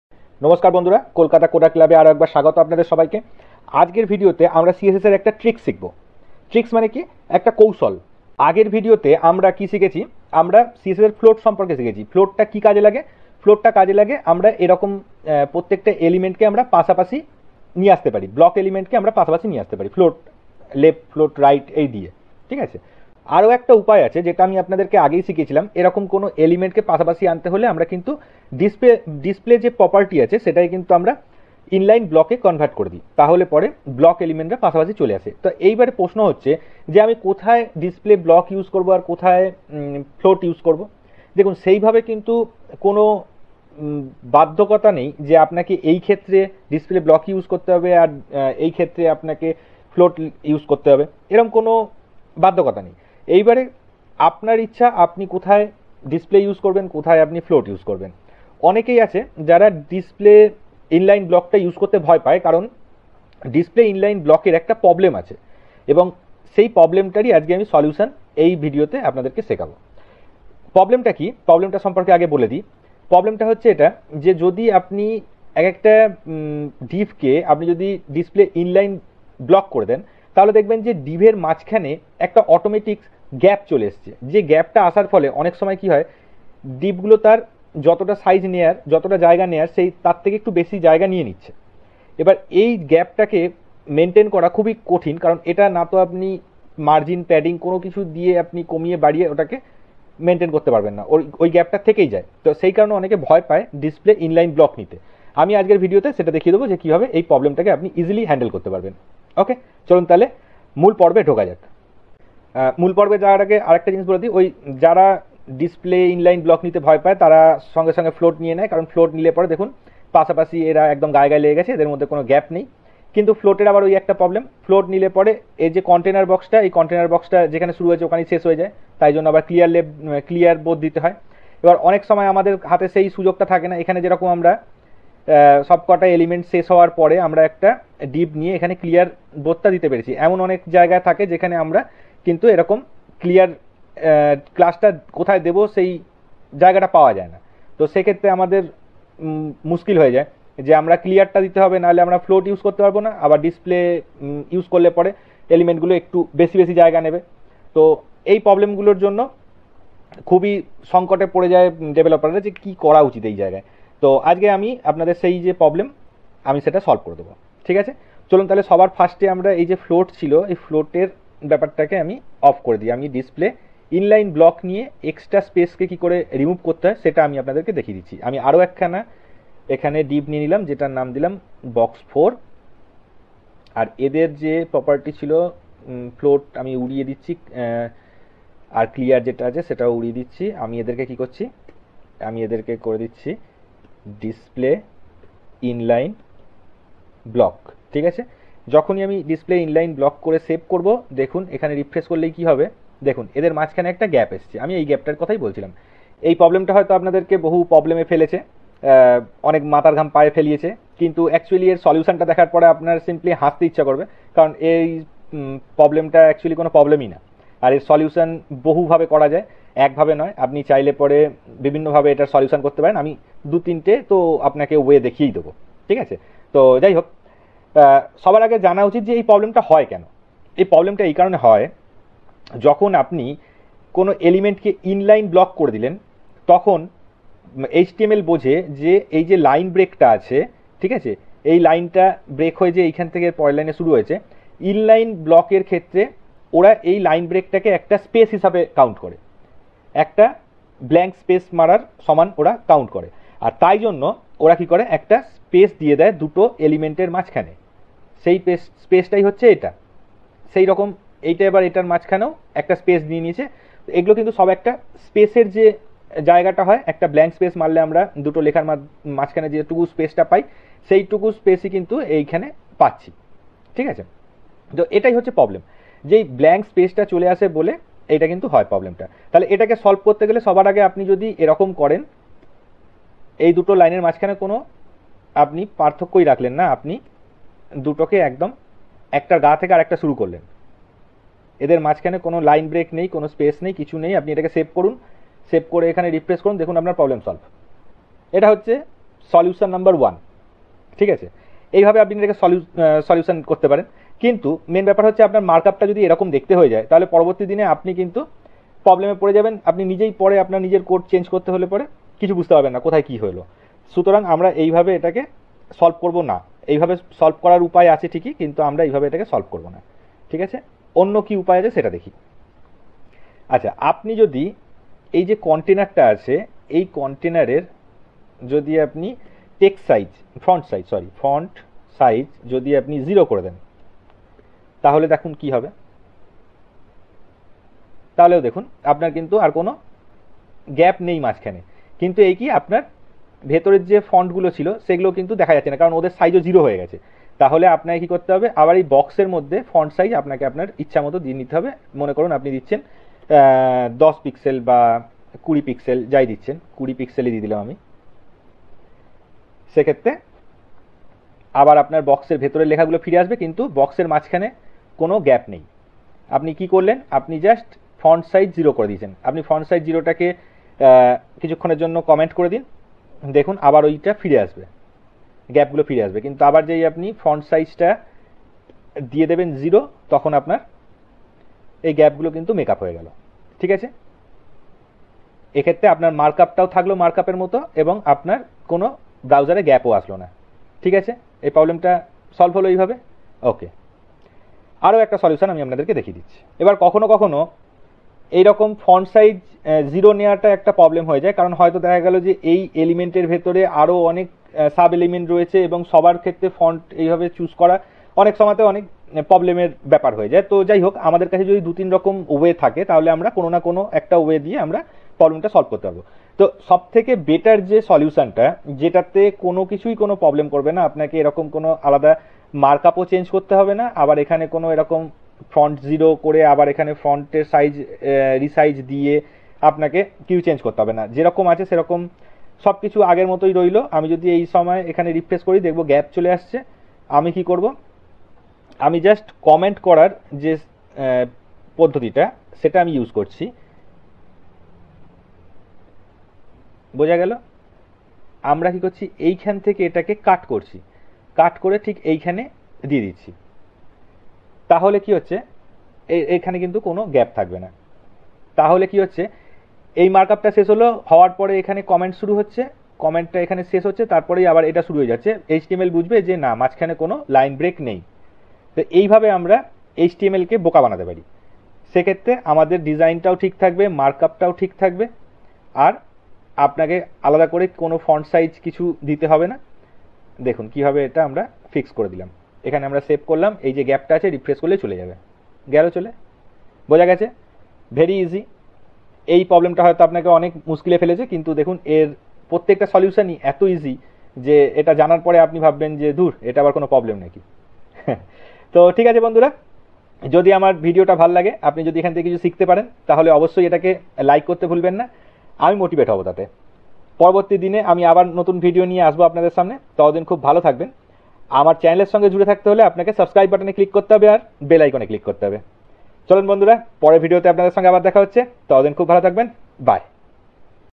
How to Remove Space Between Inline Block Elements | অতিরিক্ত গ্যাপ ঠিক করা inline এলিমেন্টএ | EP 31
In this video, I explained how to fix unnecessary space in inline-block elements, I teach multiple ways to fix this issue. Use any of them and enjoy. Happy hacking.